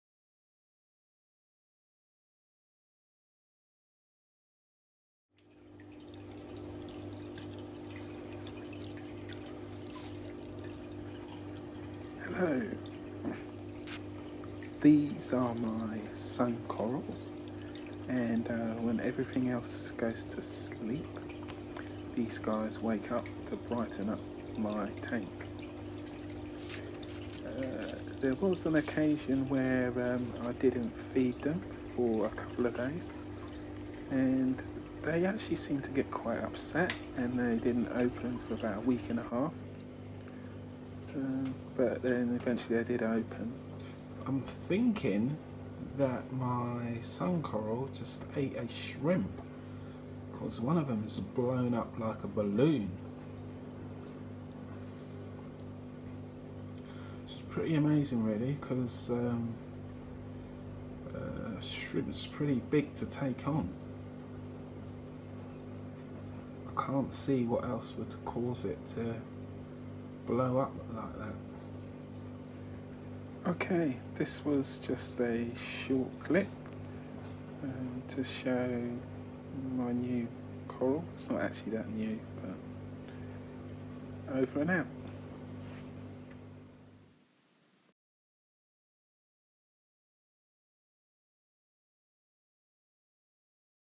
sun Coral
The Sun coral needs care and attention. I now feed them on a daily basis to keep them happy.